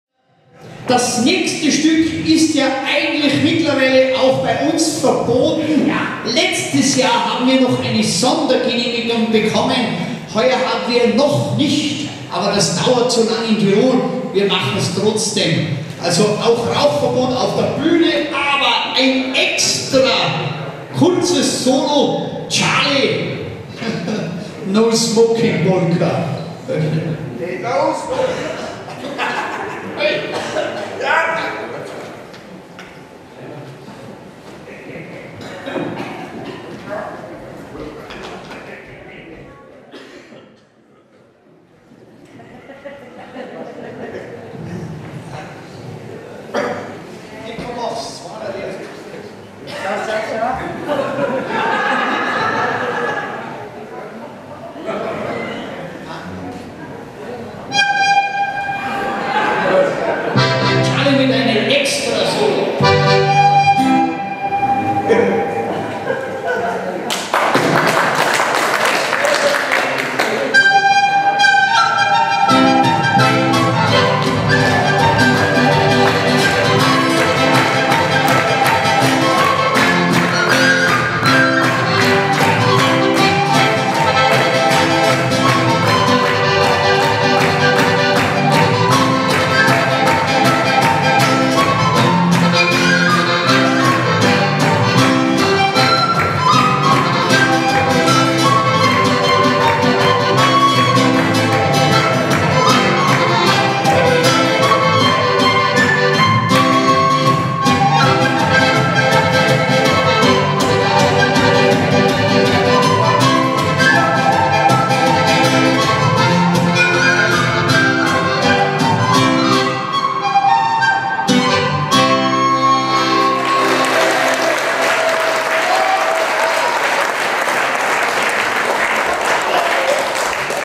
"Alpenspektakel" Blockflöten Show
Das Auner "Alpenspektakel" hat einen Livemitschnitt vom Alpenspektakelprogramm gemacht. Peter Aschaber und sein Alpenspektakel Team
bedankt sich recht herzlich beim Filmklub Fieberbrunn für die gute Zusammenarbeit.
Kamera: Leo Waltl, Elfriede Hengl, Heinz Jöbstl, Wolfgang Schwaiger, Franz Eberl;
Schnitt:  Franz Eberl